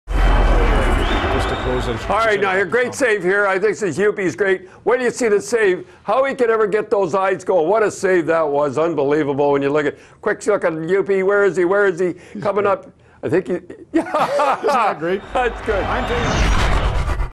Youppi's Reaction on Bishop's Save - Round 2 / Game 1 - Tampa Bay Lightning vs Montreal Canadiens
Youppi's Reaction on Ben Bishop's Save - Round 2 / Game 1 - Tampa Bay Lightning vs Montreal Canadiens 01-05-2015